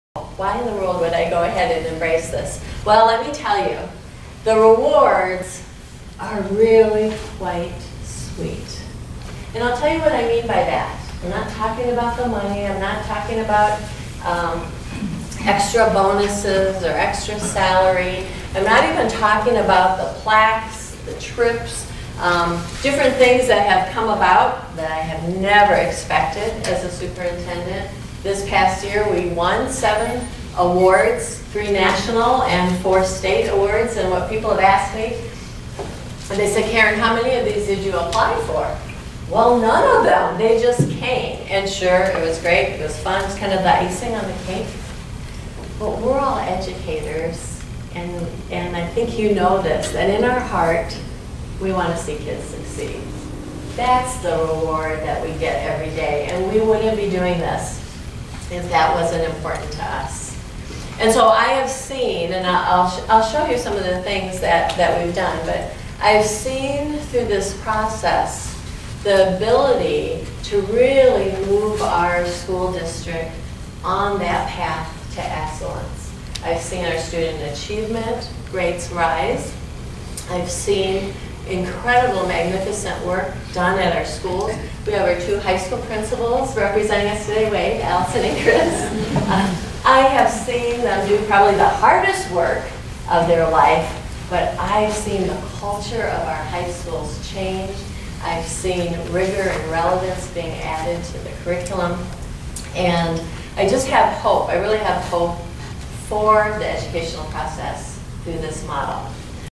"Sweet Rewards" by Living Evidence-Based Leadership
Dr. Karen Schulte, Superintendent of the School District of Janesville (WI), shares the "sweet rewards" that followed the district's implementation of the Evidence-Based Leadership framework.